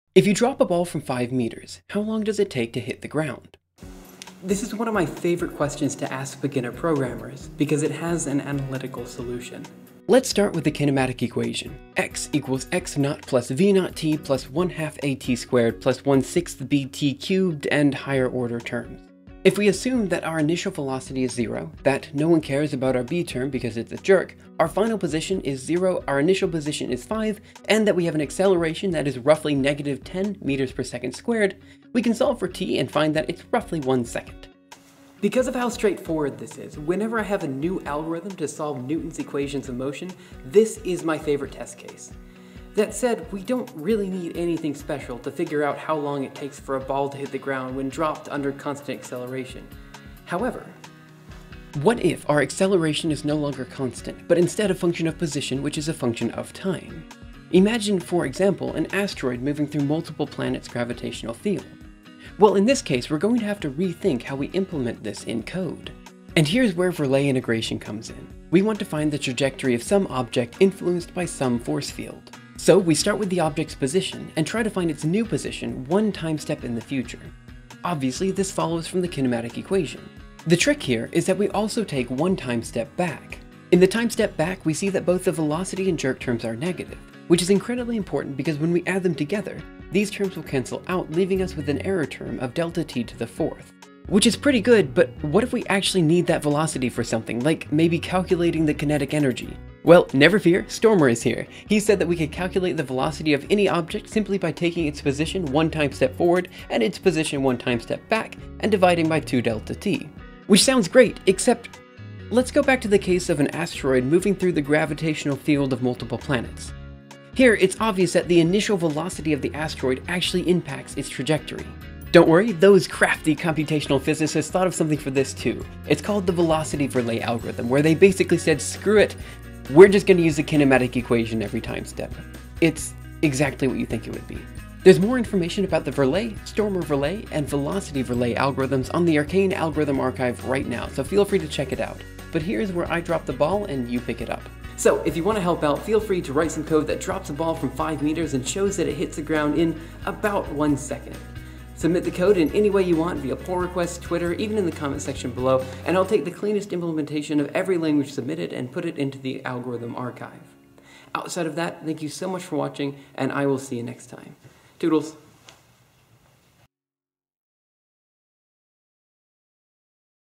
Verlet Integration
Here's a video describing a simple method to solve Newton's equations of motion.

Also: Special thanks to butt4cak3 for the thumbnail!